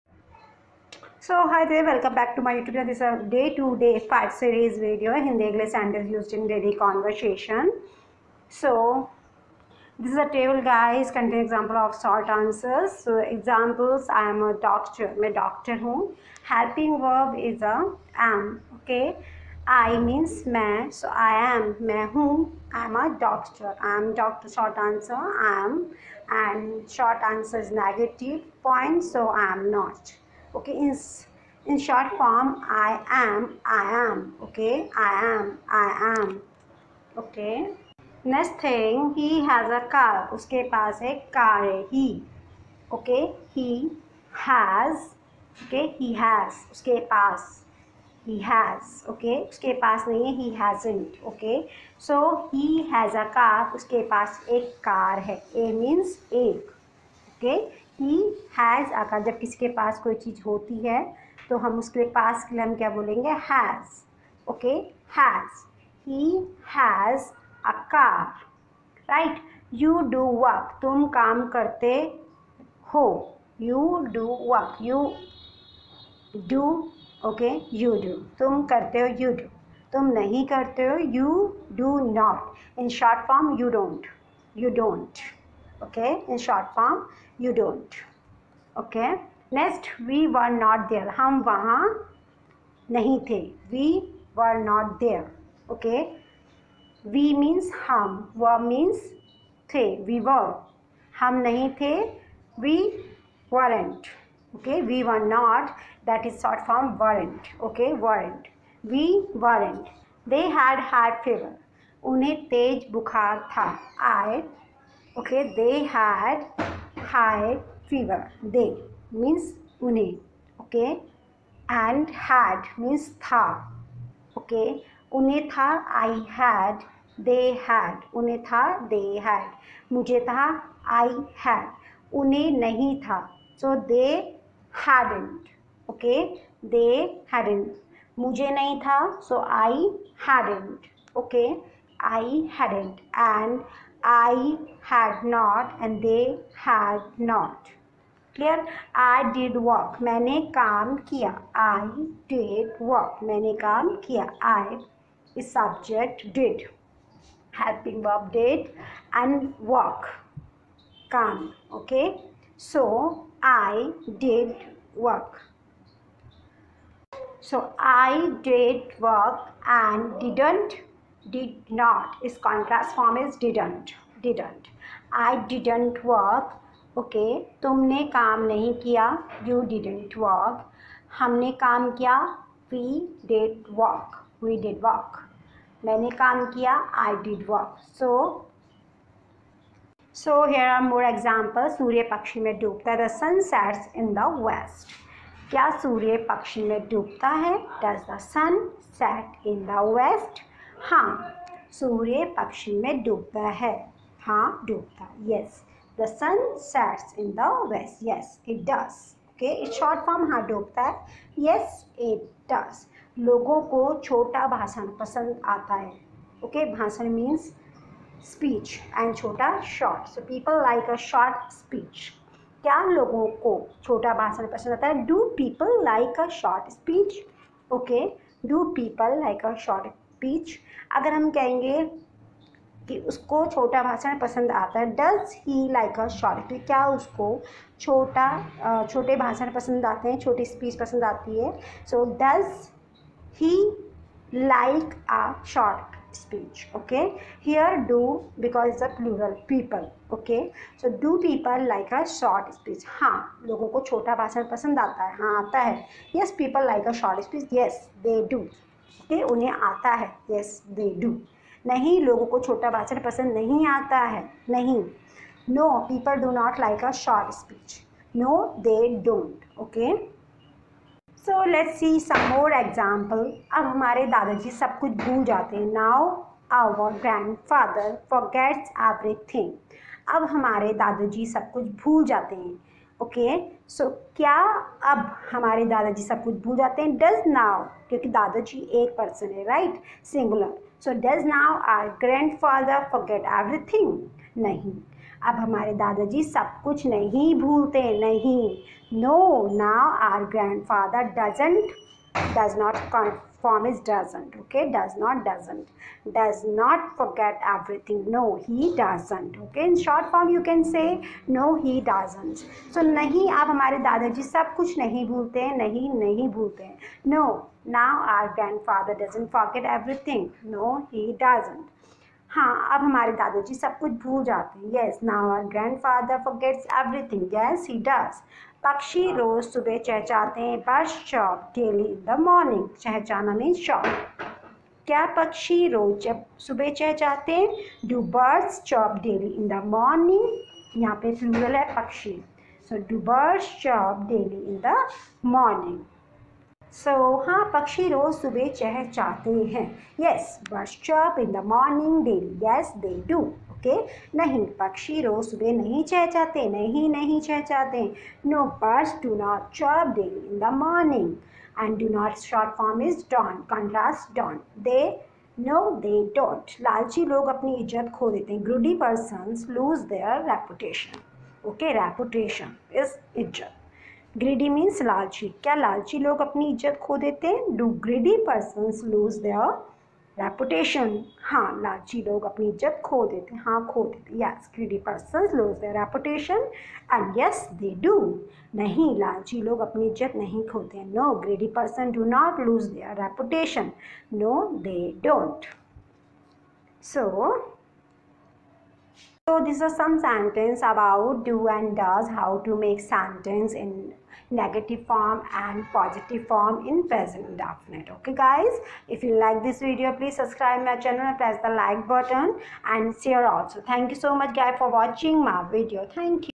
Day to day -5 how to make sentences in Present indefinite use do, does and more sentences!
how to make sentences !